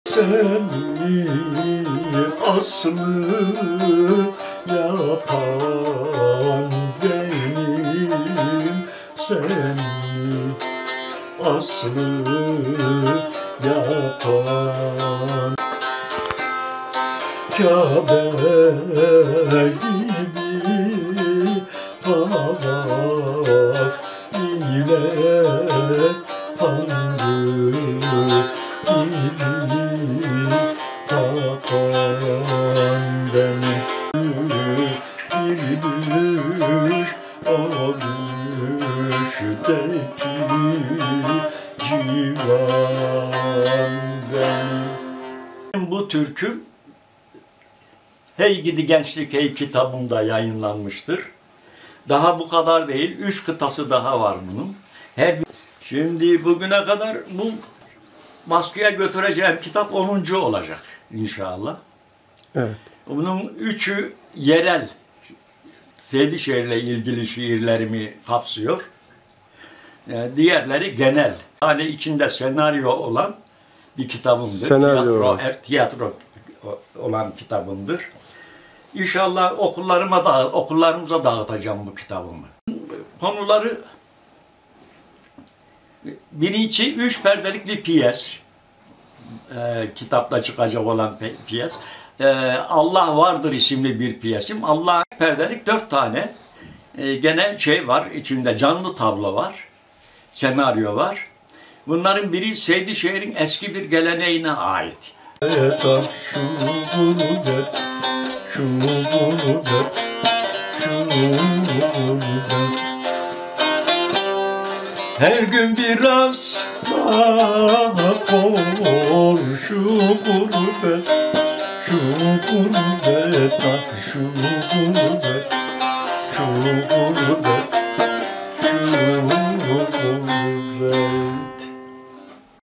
Bahaddin Paslı'nın sesinden ezgiler
Konya’nın Seydişehir  İlçesinde  yaşayan Şair,Bestekar ve  Ozan Bahaddin Paslı’nın onuncu  kitabı çıkıyor.